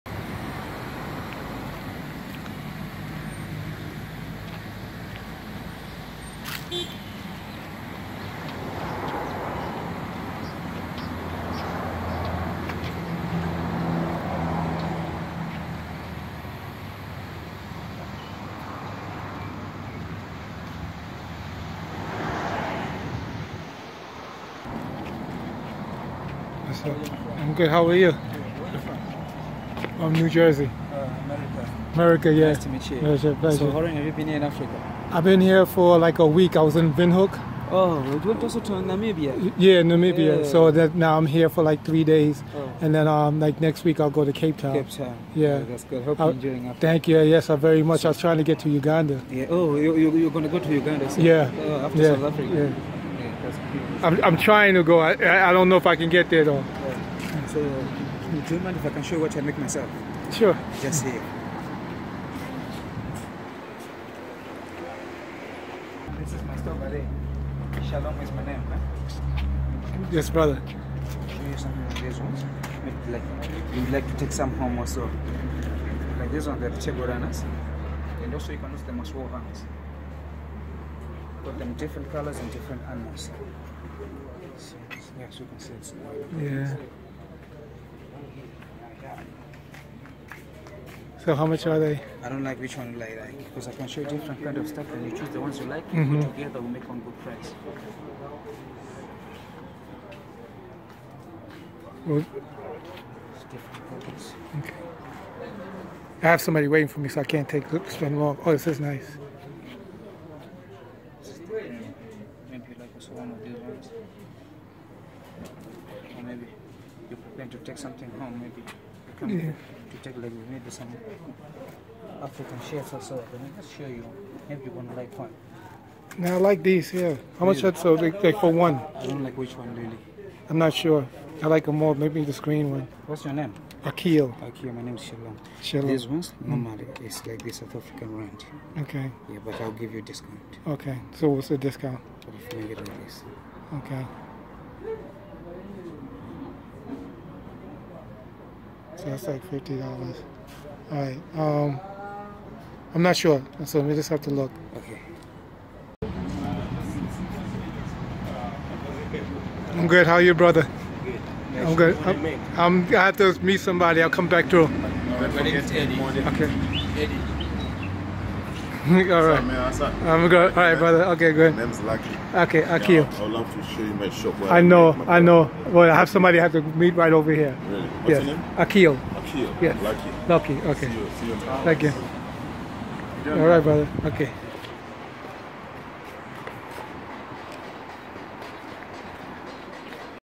Stopping At Trade Market On The Way To The Pilanesberg Game Reserve
On my way to the Pilanesberg Game Reserve  I, Aqiyl Aniys, stopped at a trade market to check out some goods. The brothers were killing me with kindness. I had to run before they enticed me to spend all my money